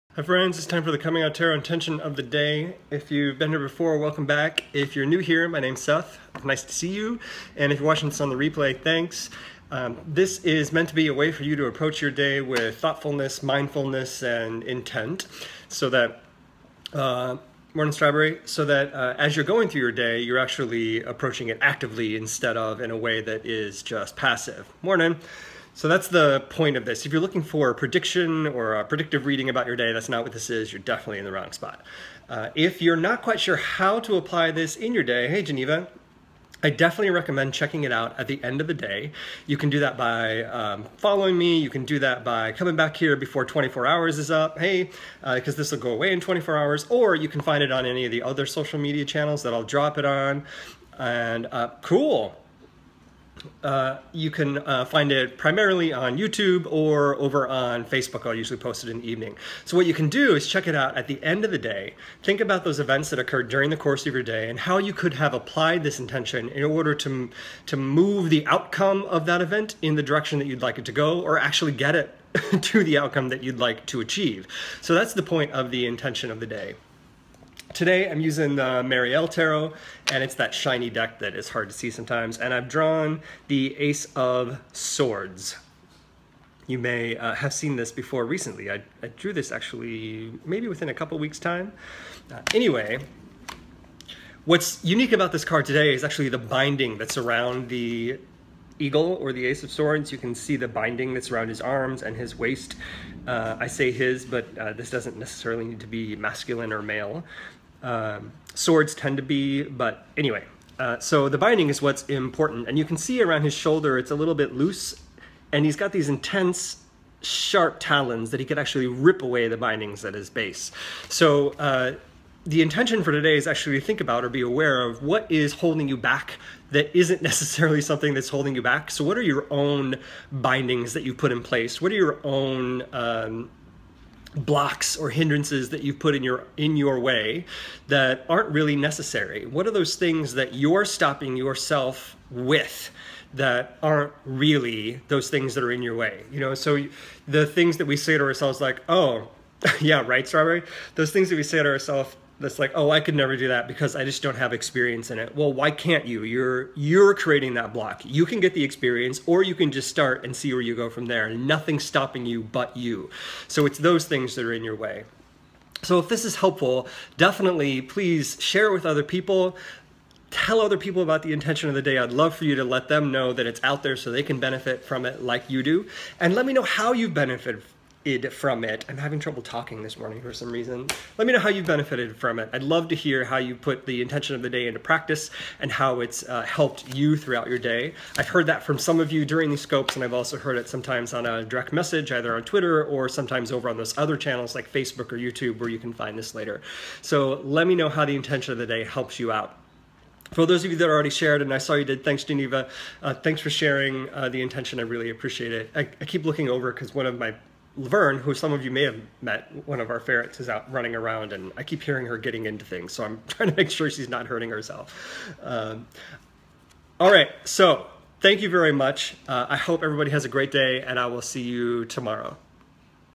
Tarot Intention of the Day - Ace of Swords
Comingouttarot.com tarot intention of the day inspires you to release the ties that bind! You can do it!